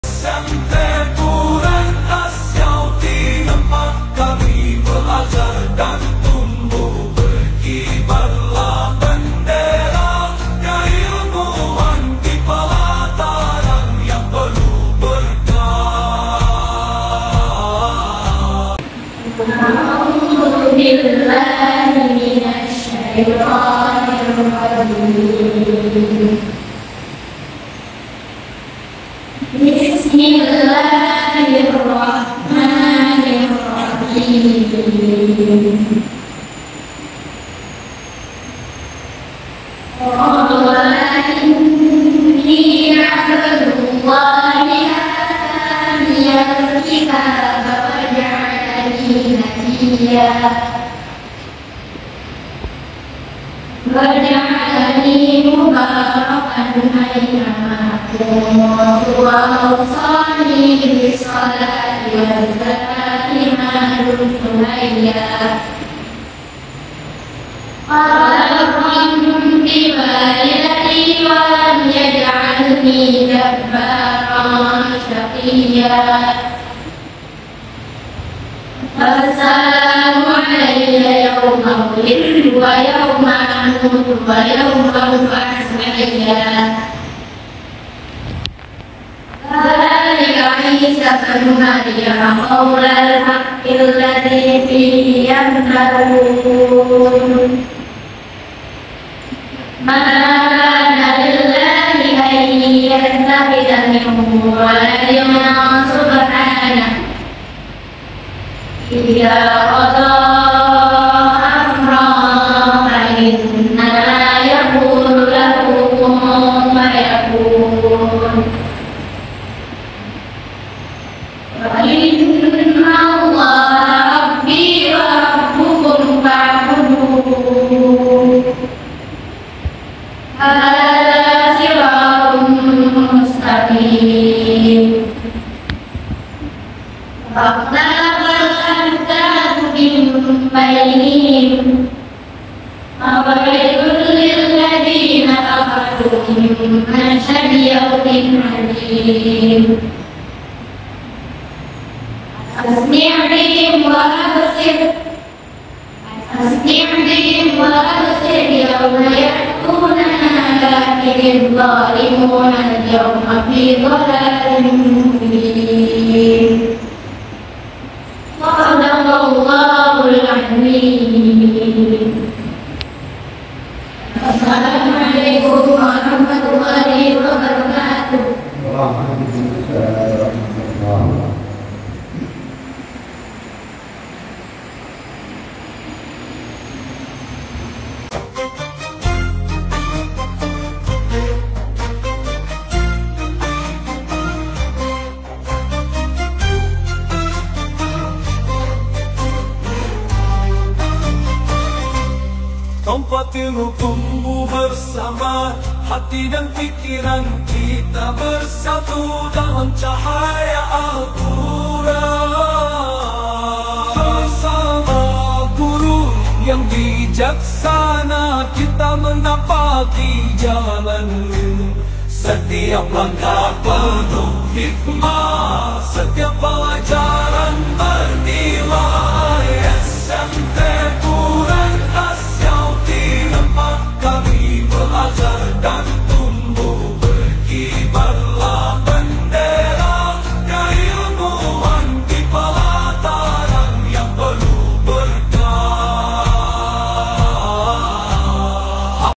Lantunan Ayat Suci Al Qur'an pada Acara Sertijab dan Restruktur Yayasan Barokah Al Qur'an Assyauqi.
Lantunan Ayat Suci Al Qur'an pada Acara Sertijab dan Restruktur Yayasan Barokah Al Qur'an Assyauqi.(Selasa, 01/10/2024)